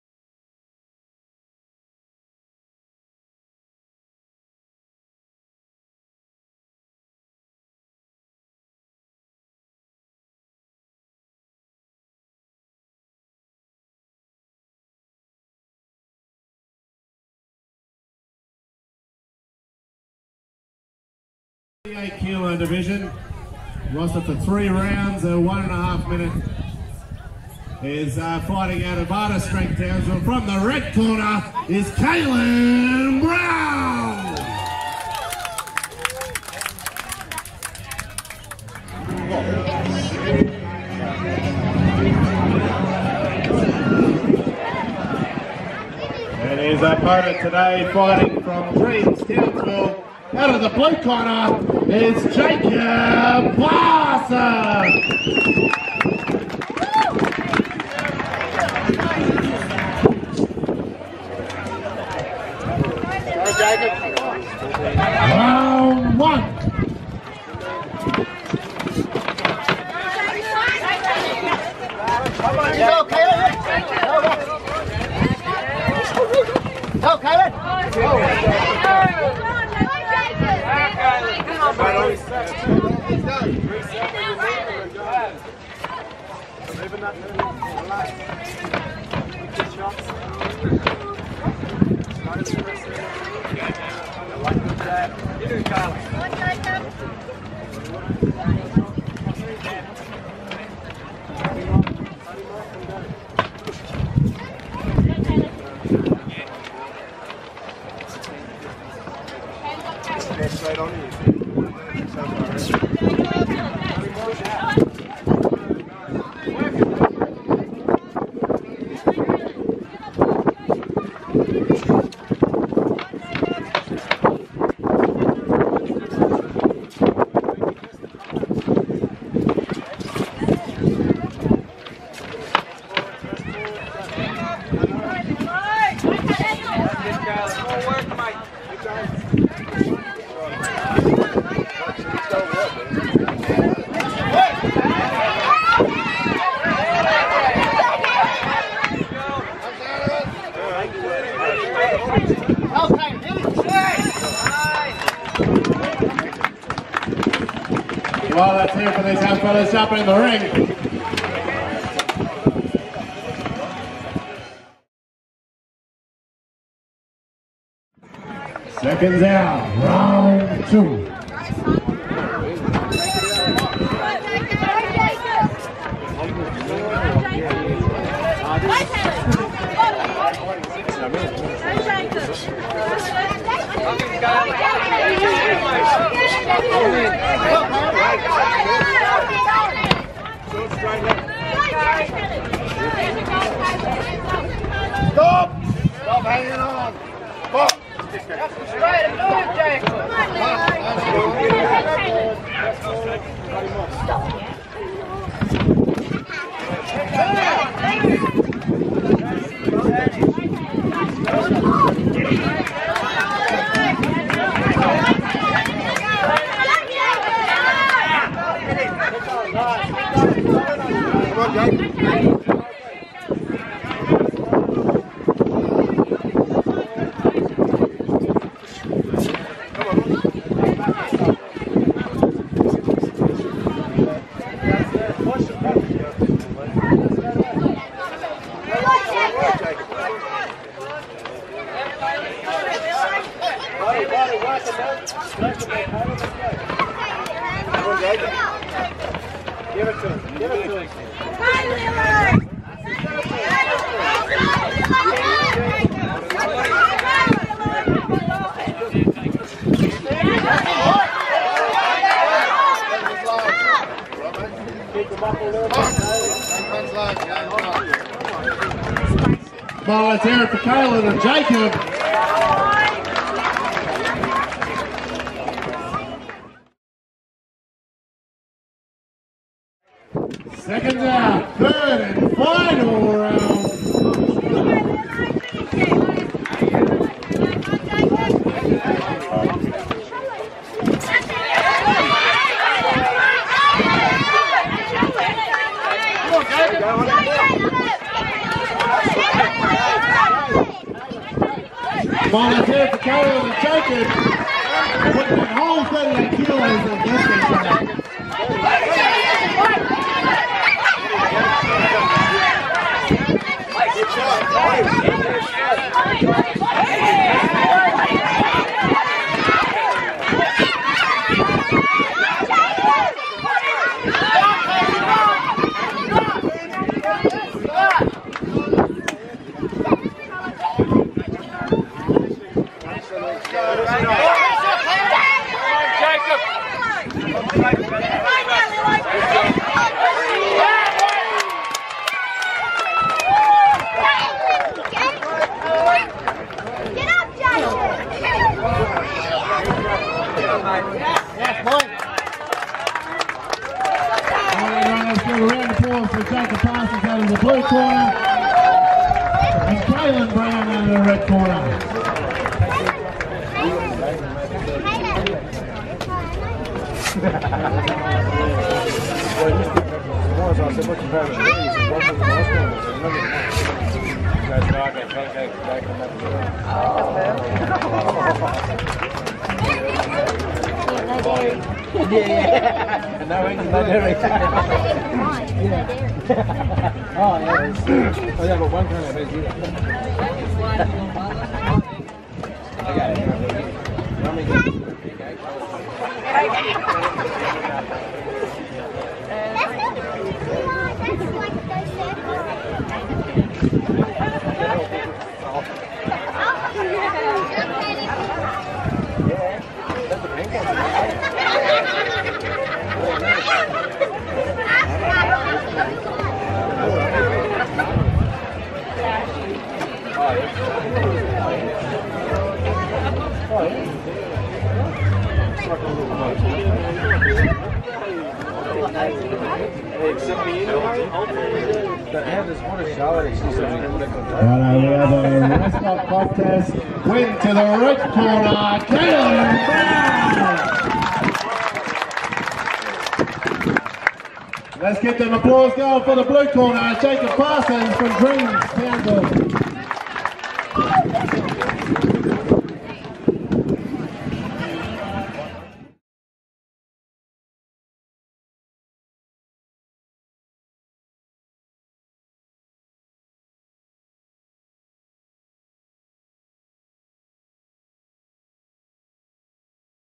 Khalon Brown vs Jacob Parsons - Ashley Cupitt Boxing Academy video
Khalon Brown [Red corner] of Art of Strength Boxing Club vs Jacob Parsons [Blue corner] of Dreams Boxing Club.
10th September 2022 Art of Strength Tournament at Hugh Street Rugby Grounds, Currajong, Townsville, QLD, Australia.

Photos by Linda Bone Photographer and Louise Anderson-Clemence.